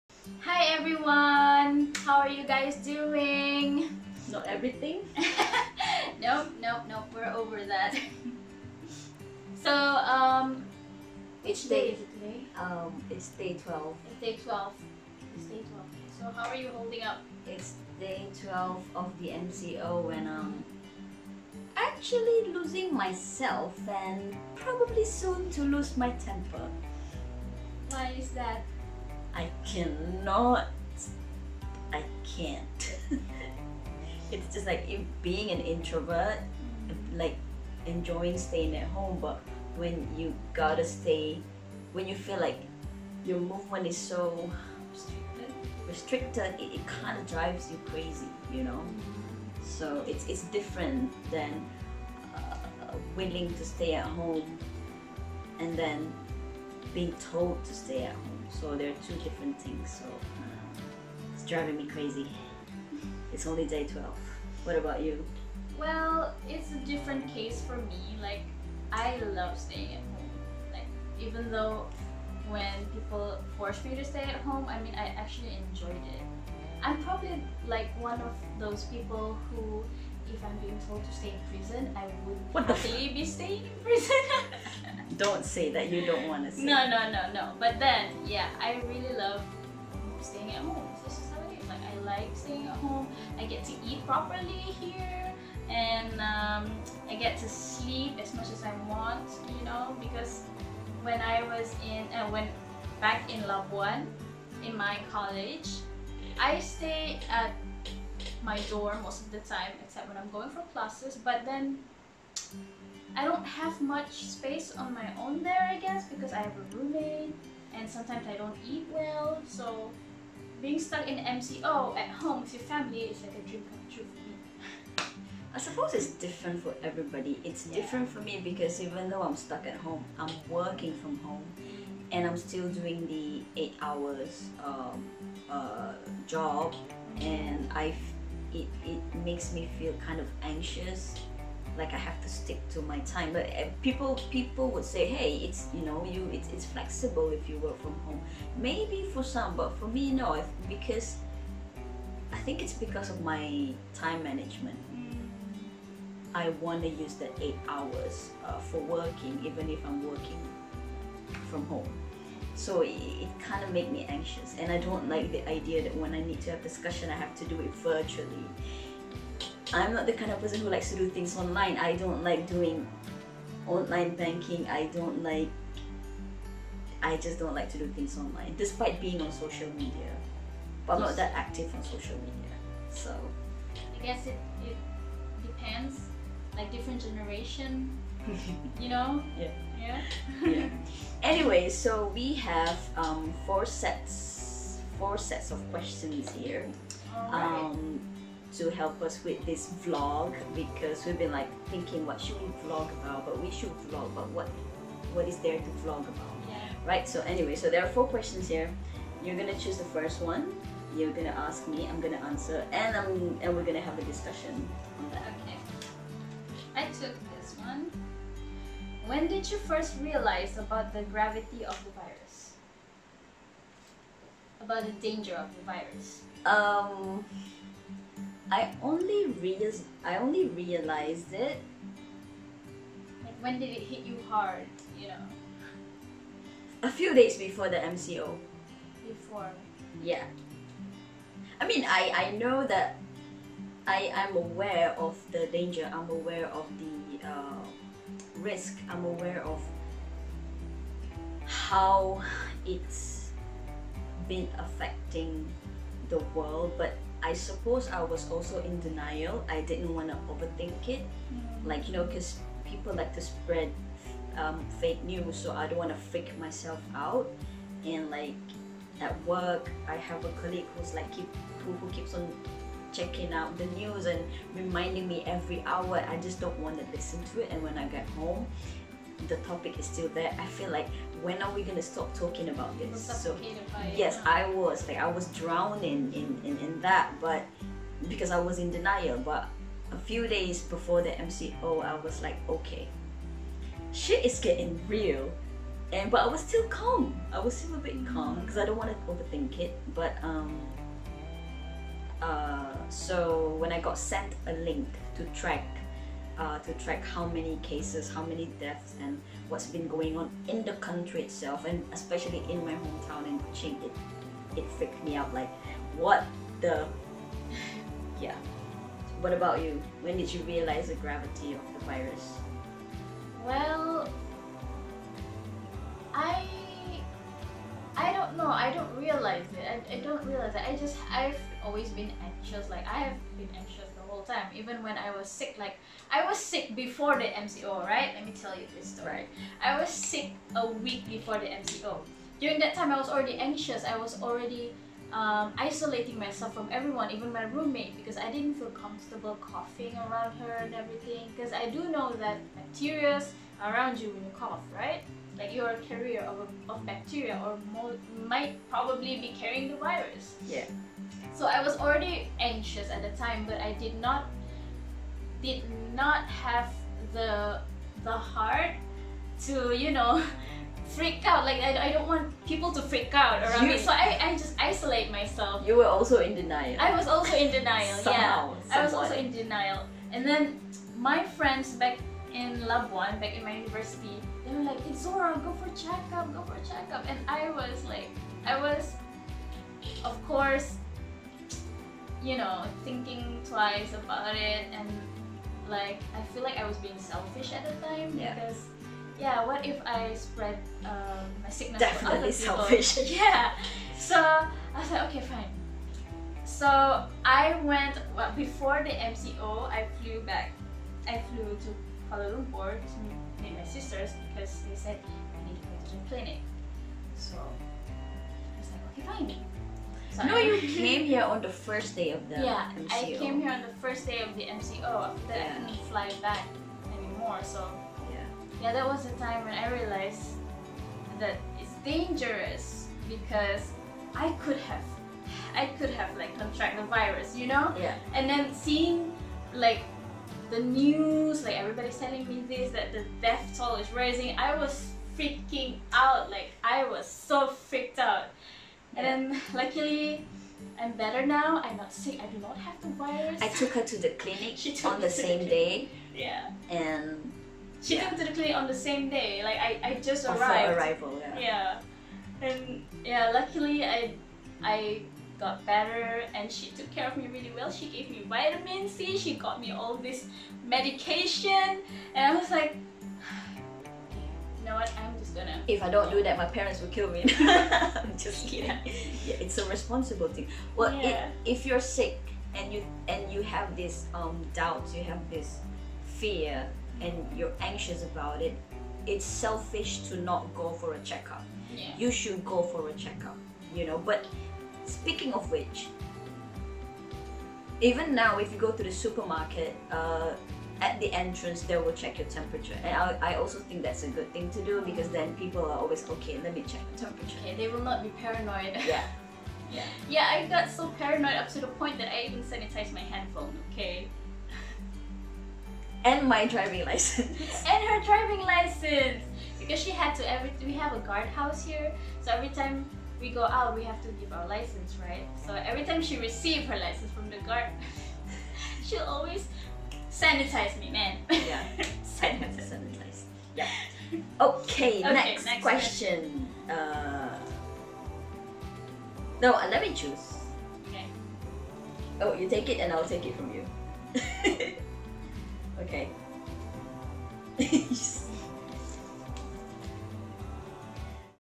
MCO Day 12 [Part 1] - When did you first realize about the gravity of the virus?
Siblings in conversation on the COVID19.